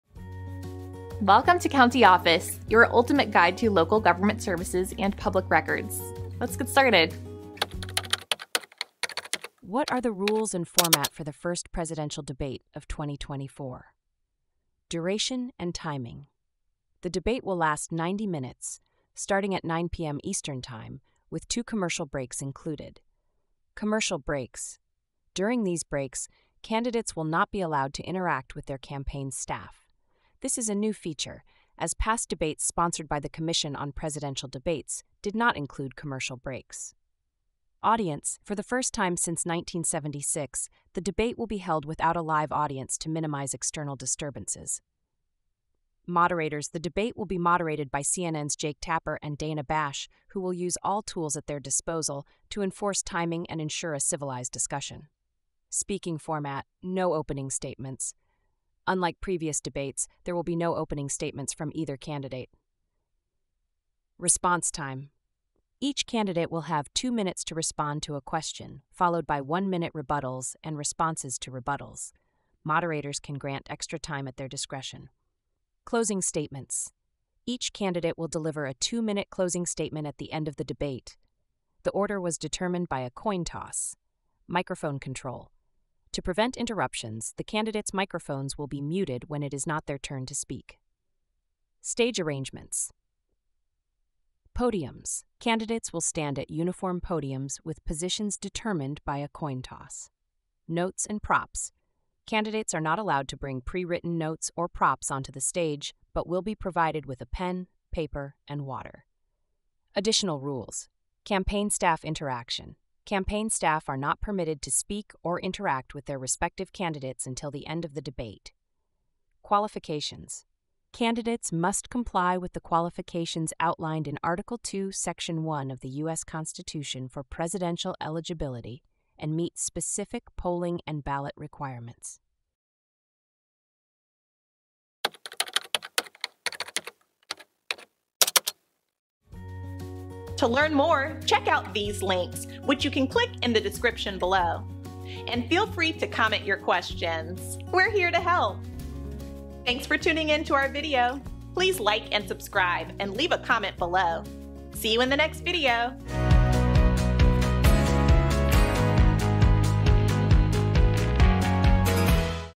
What Are the Rules and Format for the First Presidential Debate of 2024? - CountyOffice.org
What Are the Rules and Format for the First Presidential Debate of 2024? Have you ever wondered about the rules and format for the first presidential debate of 2024? In this video, we delve into the specifics of what to expect during this significant event. From the duration and timing of the debate to the speaking format and stage arrangements, we cover it all. Learn about the moderators overseeing the debate and the new features being introduced, such as commercial breaks and audience restrictions. Discover how microphone control will be implemented to ensure a structured discussion and how candidates are required to adhere to specific rules throughout the debate. Stay informed on the qualifications candidates must meet and the limitations on campaign staff interaction.